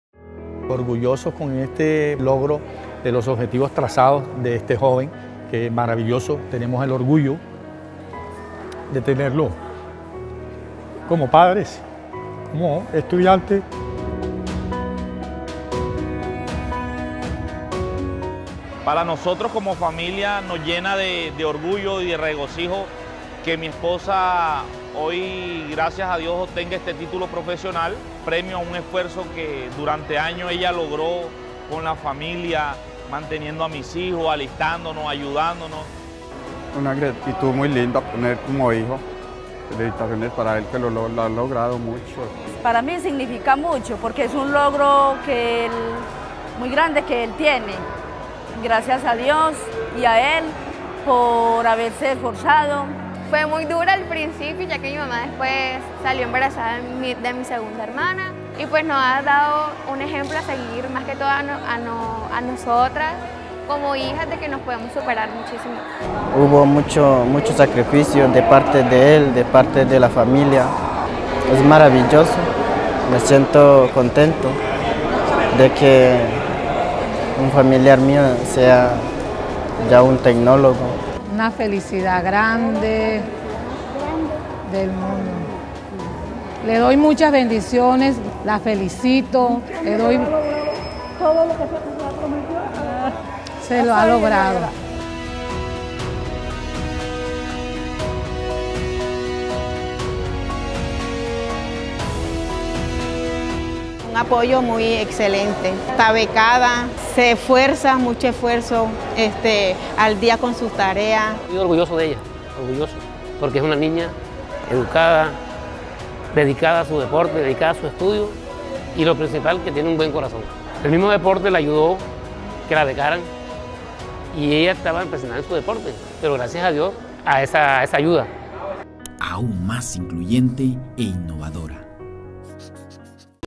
Orgullo y satisfacción entre familias de graduados del CREO por el logro de sus seres queridos
Momentos de emoción se vivieron durante las recientes ceremonias de graduación de la Universidad del Magdalena, a través del Centro para la Regionalización de la Educación y las Oportunidades – CREO; la presencia de familiares y amigos evidenció la importancia de la educación para el futuro de los jóvenes. Las lágrimas y los aplausos no se hicieron esperar; para ellos, ver a sus seres queridos culminar una etapa académica tan significativa fue motivo de inmensa alegría.